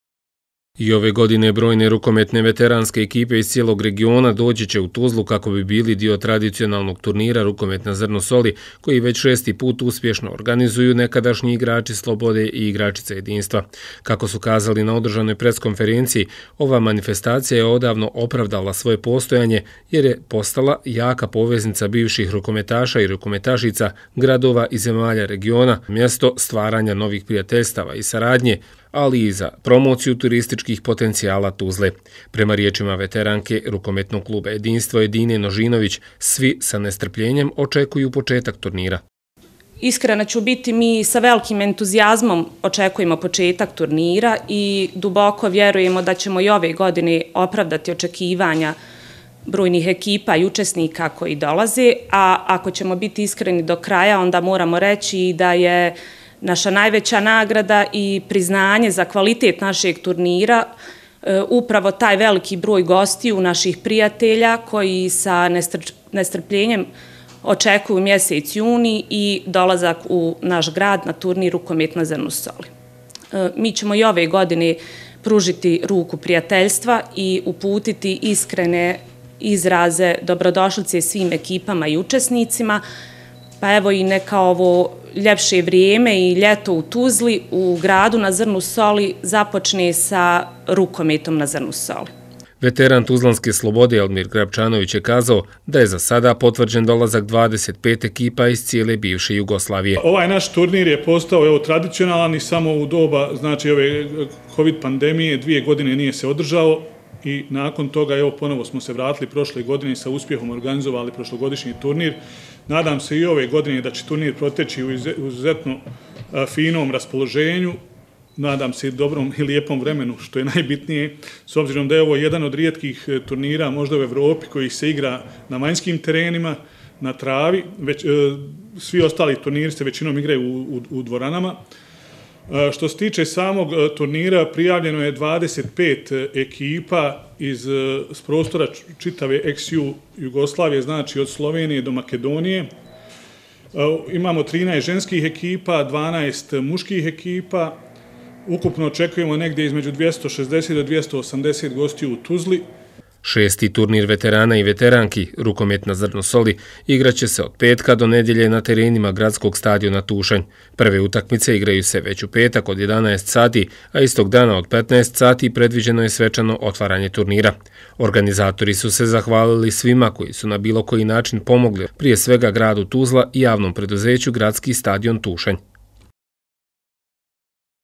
U petak počinje tradicionalni turnir veteranki i veterana "Rukomet na zrnu soli"
Od petka do nedjelje na terenima Gradskog stadiona Tušanj biće održan turnir veteranki i veterana “Rukomet na zrnu soli”, o čemu je više riječi bilo na današnjoj press konferenciji.

(RTV7)